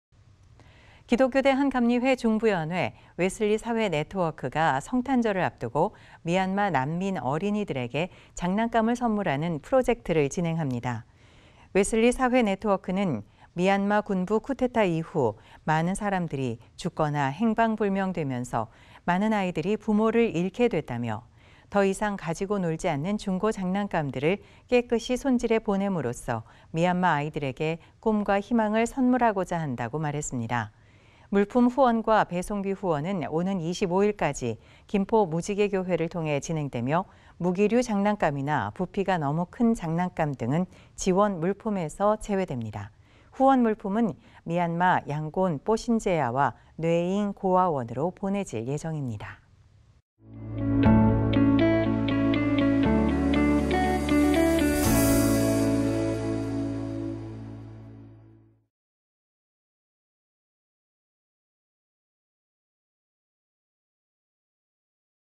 [CBS 뉴스] 웨슬리사회네트워크, 미얀마 난민 장난감 지원 프로젝트
기독교대한감리회 중부연회 웨슬리사회네트워크가
성탄절을 앞두고
미얀마 난민 어린이들에게 장난감을 선물하는
프로젝트를 진행합니다.

웨슬리사회네트워크는
"미얀마 군부 쿠데타 이후
많은 사람들이 죽거나 행방불명되면서
많은 아이들이 부모를 잃게 됐다"며
"더 이상 가지고 놀지 않는 중고 장난감들을
깨끗이 손질해 보냄으로써
미얀마 아이들에게 꿈과 희망을 선물하고자 한다"고 말했습니다.

물품 후원과 배송비 후원은
오는 25일까지 김포 무지개교회를 통해 진행되며,
무기류 장난감이나 부피가 너무 큰 장남감 등은
지원 물품에서 제외됩니다.

후원 물품은 미얀마 양곤 뽀신제야와
뇌에잉 고아원으로 보내질 예정입니다.